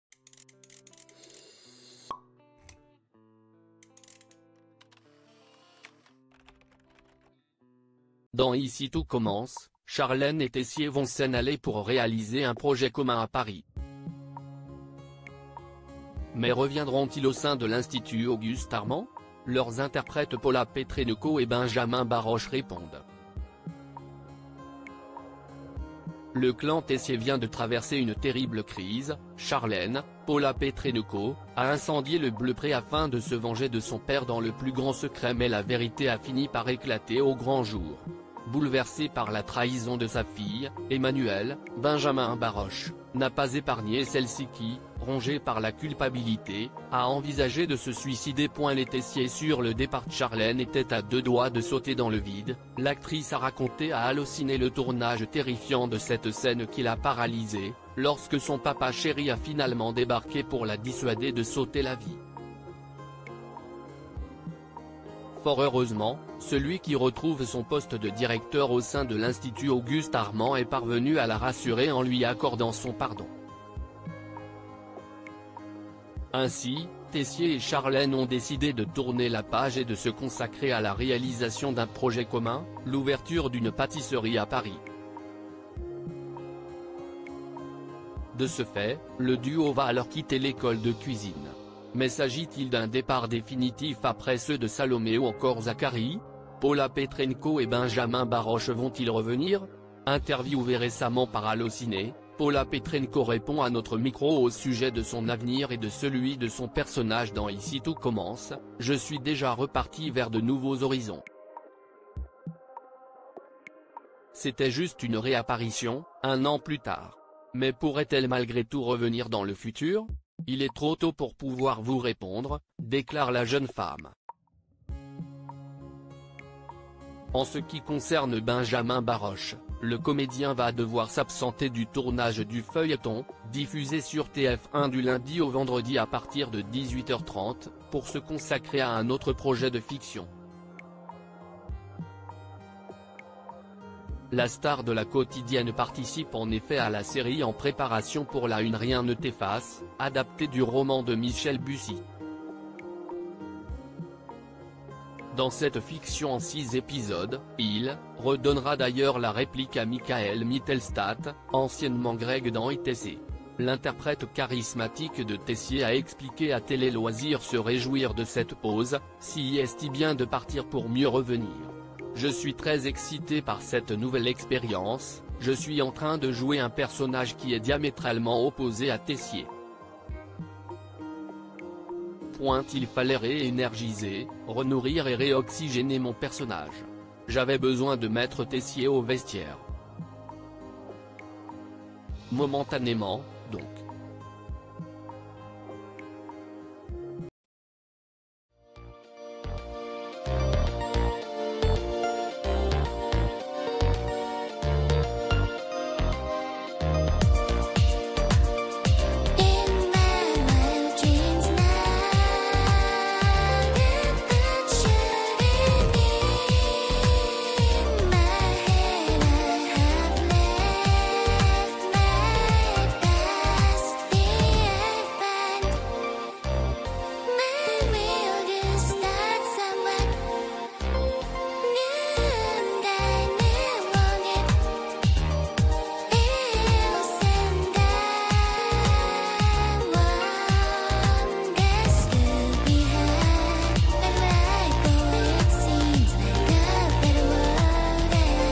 Ici tout commence : Charlène et Teyssier vont-ils revenir ? Pola Petrenko et Benjamin Baroche répond
Ici tout commence : Charlène et Teyssier vont-ils revenir ? Pola Petrenko et Benjamin Baroche répondent